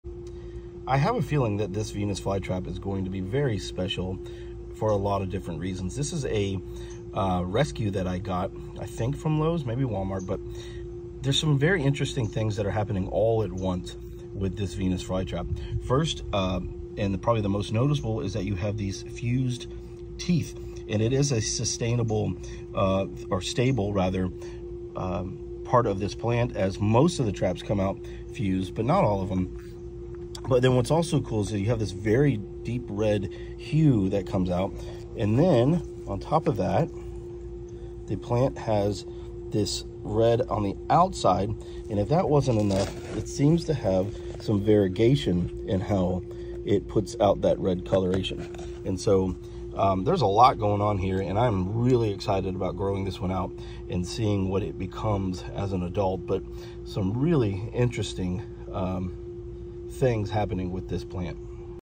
This Venus flytrap has all kinds of interesting traits!
This Venus flytrap has some crazy traits. It produces a multitude of oddities.